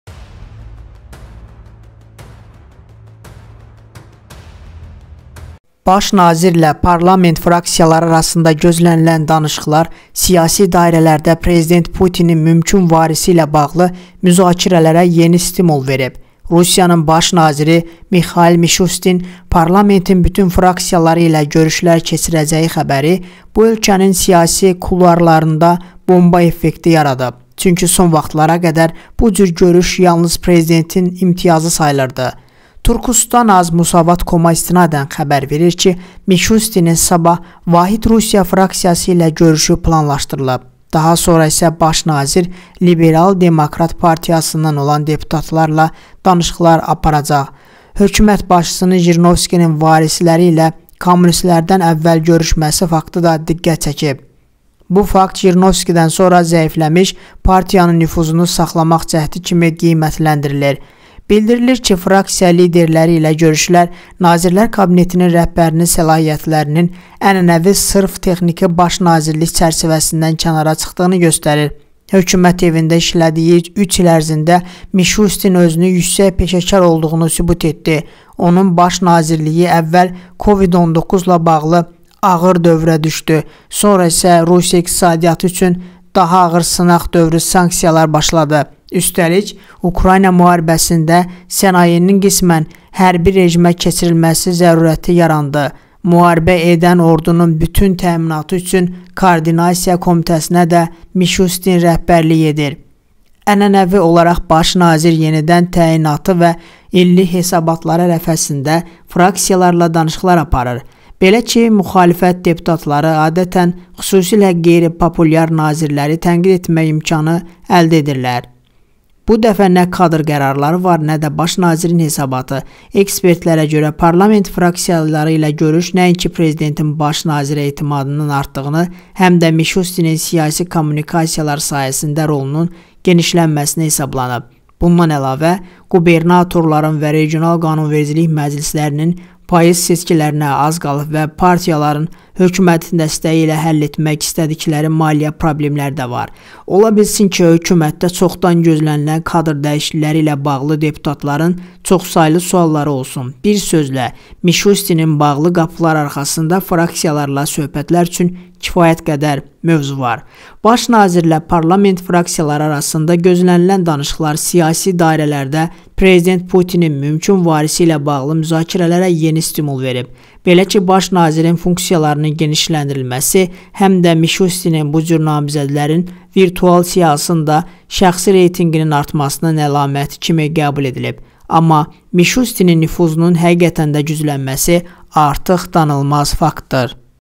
Putini əvəz edəcək adamın ADI MƏLUMDUR – Bu şəxs əsas siyasi fiqura çevrilir
Turkustan.az Musavat.com-a istinadən xəbər verir ki, Mişustinin sabah “Vahid Rusiya” fraksiyası ilə görüşü planlaşdırılıb, daha sonra isə baş nazir Liberal Demokrat Partiyasından olan deputatlarla danışıqlar aparacaq. Hökumət başçısının Jirinovskinin varisləri ilə  kommunistlərdən əvvəl  görüşməsi faktı da diqqət çəkib. Bu  fakt Jirinovskidən sonra zəifləmiş  partiyanın nüfuzunu saxlamaq cəhdi kimi qiymətləndirilir.